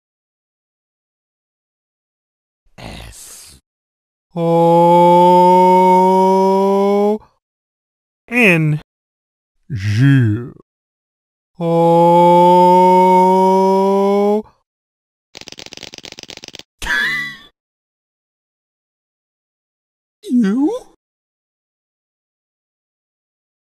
"Son Goku" | ALPHABET LORE
Using "Alphabet Lore", we will animate the words you want.
If you have many requests, we will randomly make it.